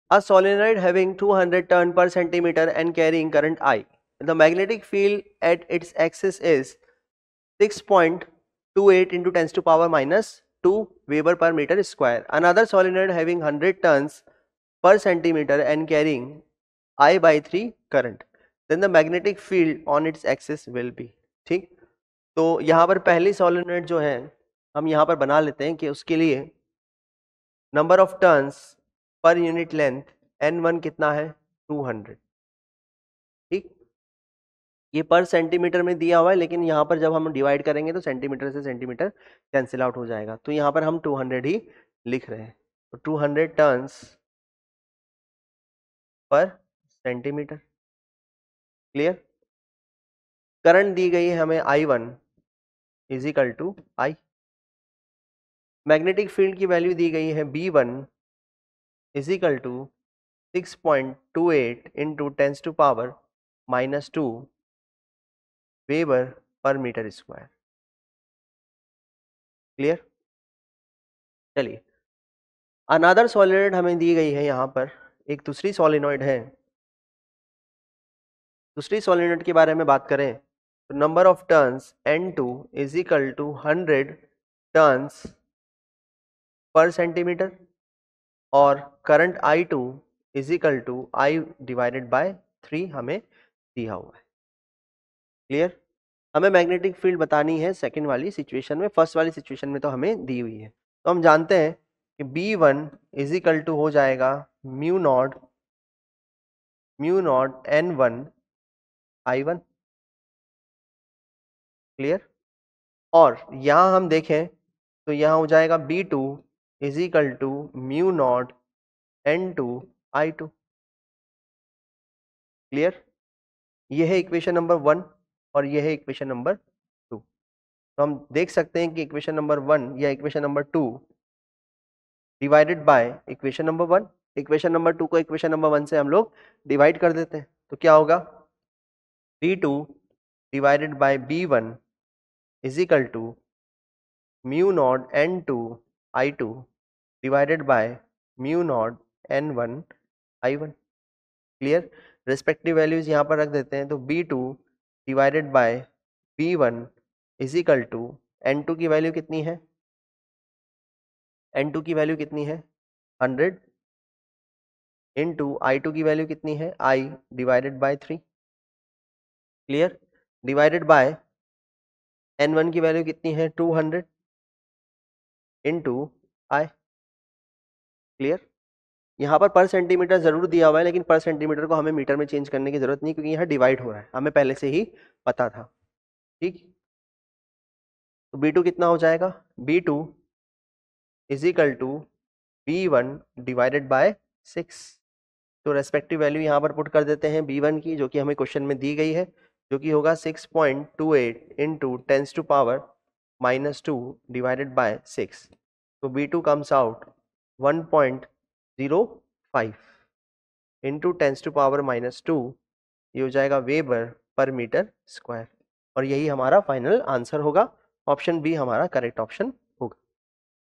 A long solenoid having 200 turns/cm and carries current i. Magnetic field at its axis is 6.28 &t....
A long solenoid having 200 turns/cm and carries current i. Magnetic field at its axis is 6.28 &times; 10-2 wb/m&sup2;. Another solenoid having 100 turns/cm and carries&nbsp;i3 &nbsp;current, then magnetic field at its axis will be- 📲PW App